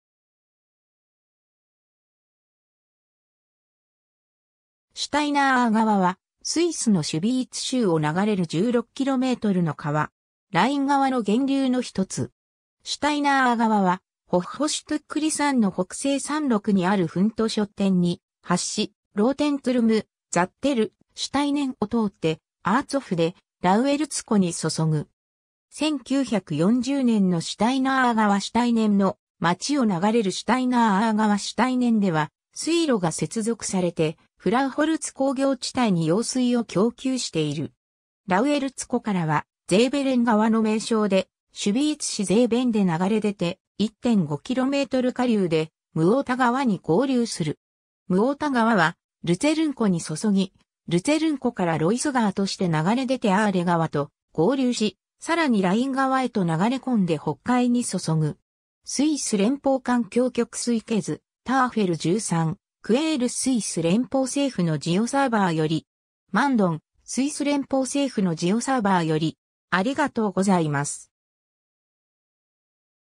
シュタイナー・アー川
シュタイナー・アー川（シュタイナー・アーがわ、独: Steiner Aa）は、スイスのシュヴィーツ州を流れる16kmの川。
ライン川の源流の一つ。
 シュタイナー・アー川は、ホッホシュトゥックリ山の北西山麓にあるフントショッテン（Hundschotten）に発し（標高1,473m）、ローテントゥルム、ザッテル、シュタイネンを通ってアーツォフ（Aazopf）でラウエルツ湖に注ぐ。
 1940年のシュタイナー・アー川 シュタイネンの町を流れるシュタイナー・アー川 シュタイネンでは水路が接続されて、フラウホルツ工業地帯（Industriezone Frauholz）に用水を供給している。
 ラウエルツ湖からはゼーヴェレン川の名称でシュヴィーツ市ゼーヴェンで流れ出て、1.5km下流でムオータ川に合流する（標高438m）。
ムオータ川はルツェルン湖に注ぎ、ルツェルン湖からロイス川として流れ出てアーレ川と合流し、さらにライン川へと流れ込んで北海に注ぐ。
 ^ スイス連邦環境局水系図 Tafel_13 ^ Quelle スイス連邦政府の Geoserver より。
 ^ Mündung スイス連邦政府の Geoserver より。